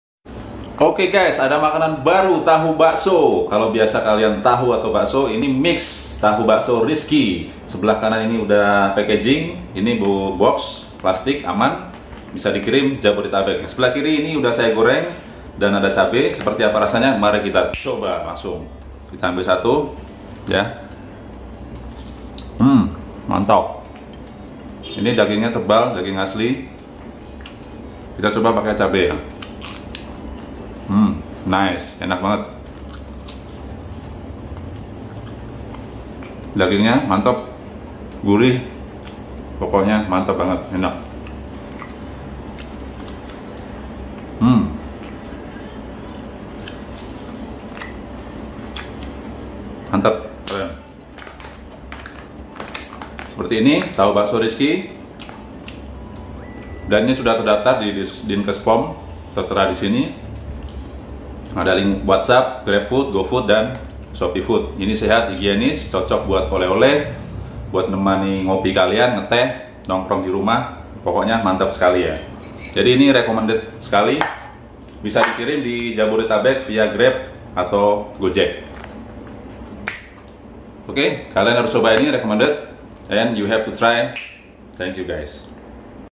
LEZATNYA...!!! TAHU BAKSO INI...GUYS...
TAHU BAKSO RIZKI

lezat tanpa pengawet & bergizi. 🙏🙏🙏
berprotein tinggi👍👍oo
dengan daging asli tebal di dalam dan segar dagingnya MANTAB...!

dikirim Gojek/Grab (Jabodetabek)